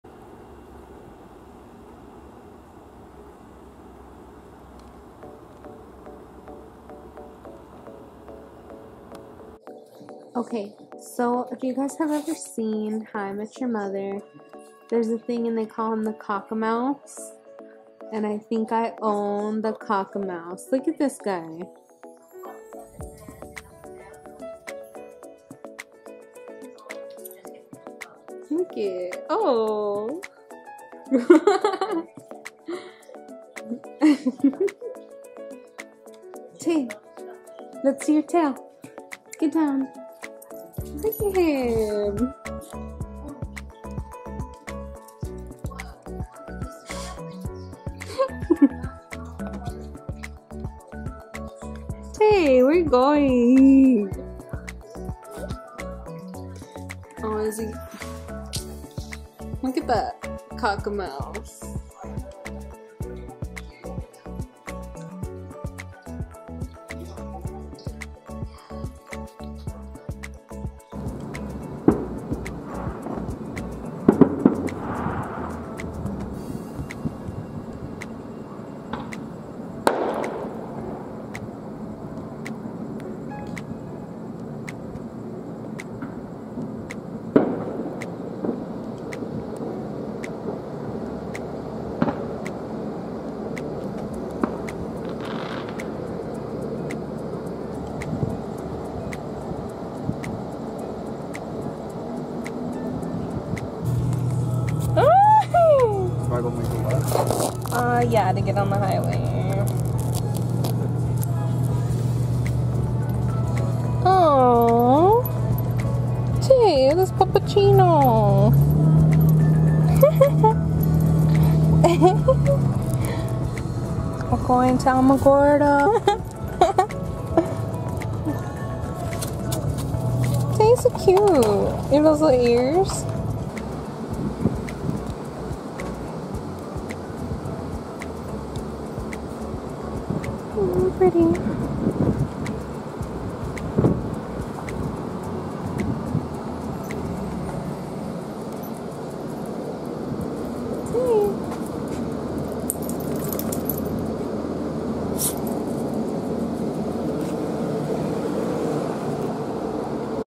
Clips of Tay & the 4th of July
This is a short video I had been planning to add to but never did so I just figured why not post it.

Gabrielle's ig: long8ody , channel.page  -  twitter : gabbriyell
Tay's ig : _taytertot__ that's 2 underscore at the end :)